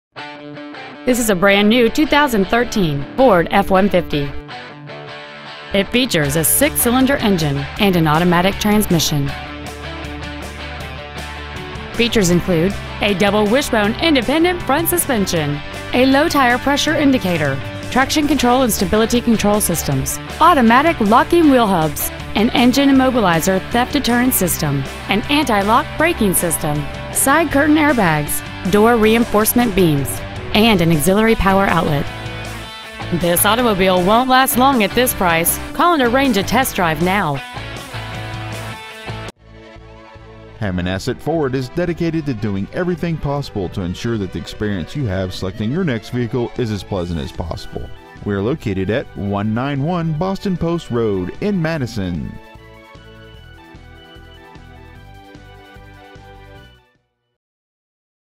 2013 Ford F-150 Madison CT
Year : 2013
 Make : Ford
 Model : F-150
 Engine : 3.5L V6 24V GDI DOHC TWIN TURBO
 Trans . : 6-SPEED AUTOMATIC
 Exterior : INGOT SILVER METALLIC
 Miles : 5
 Interior : BLACK
 Stock : N3232